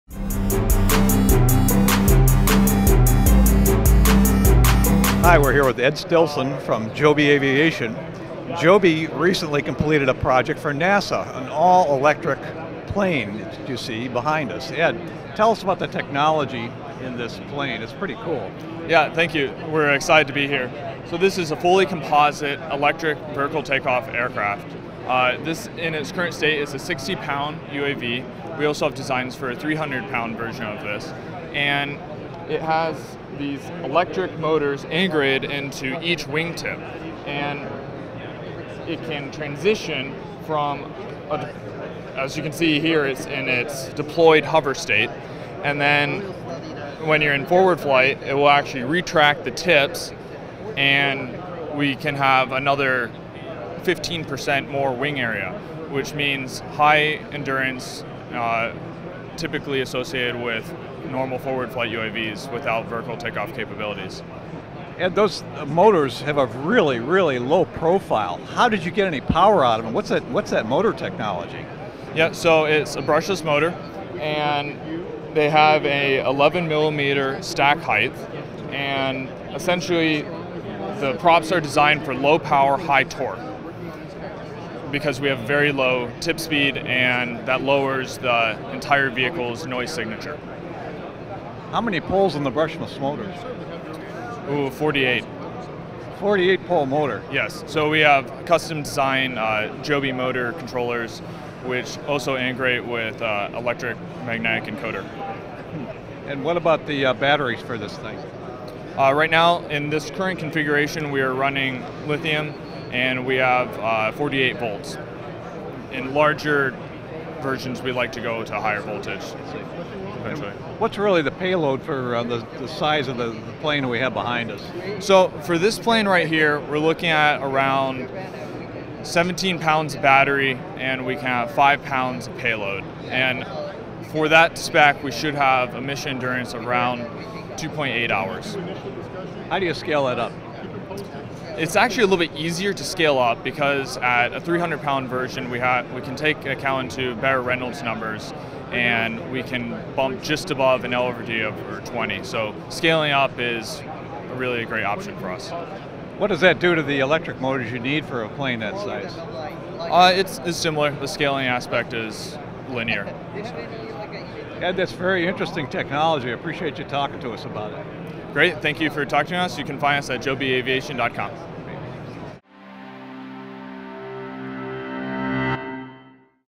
NASA looks at an advanced VTOL concept
The Lotus concept has two-bladed rotors mounted on its wingtips that provide thrust in takeoff and landing, but scissor closed in flight to become part of the wing.